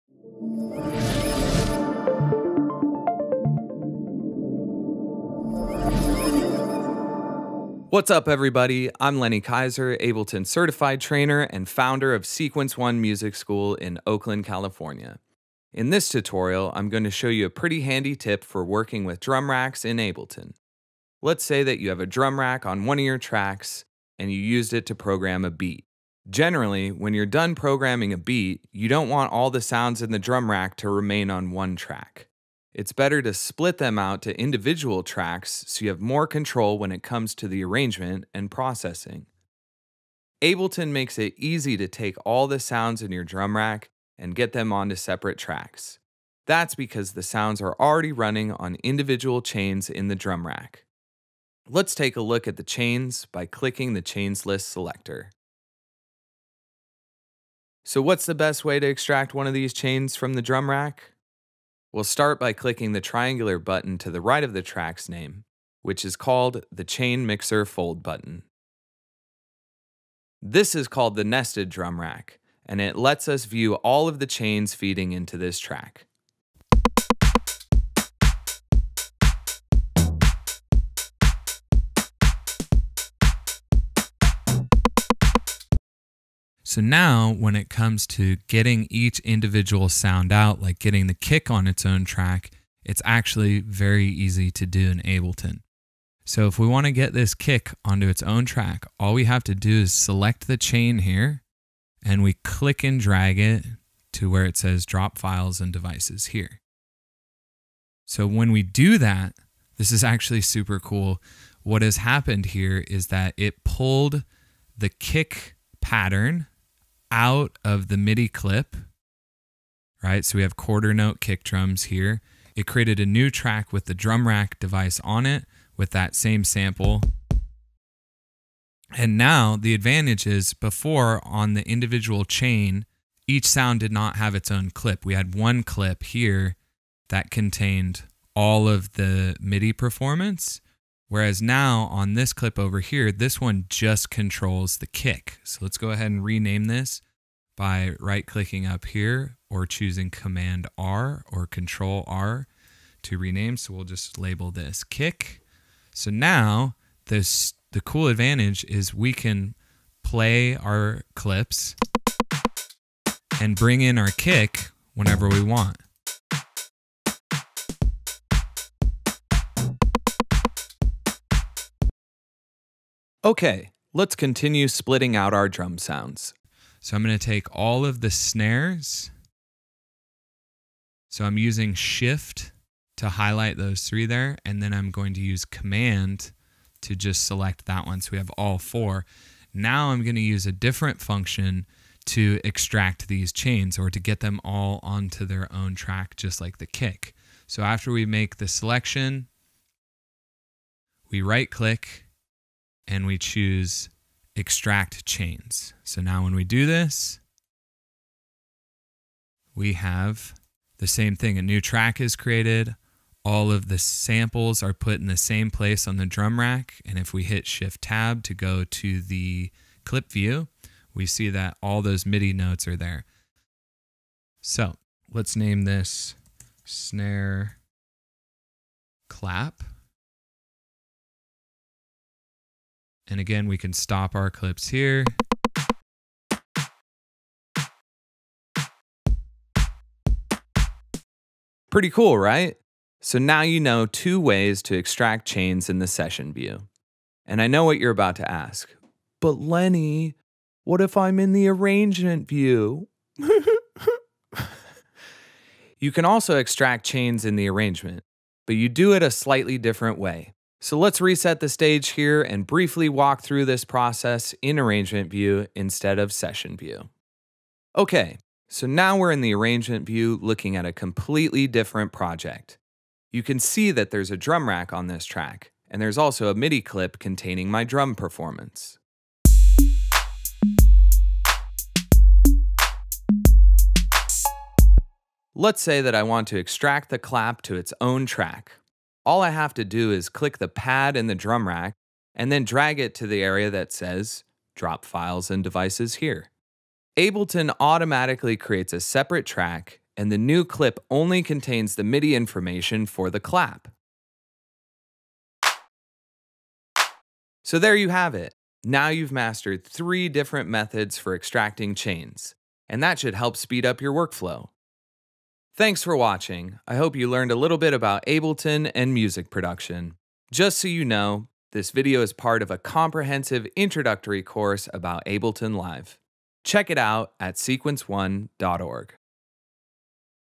Split Drum Rack Sounds to Individual Tracks - Ableton Tutorial
In this tutorial, you'll learn a pretty handy technique for working with drum racks in Ableton.